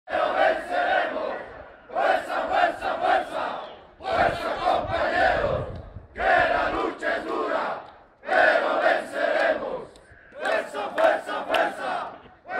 Protesta de trabajadores de Enatex
Los trabajadores exigen estabilidad laboral y cuestionan el cierre de las cuatro plantas de Enatex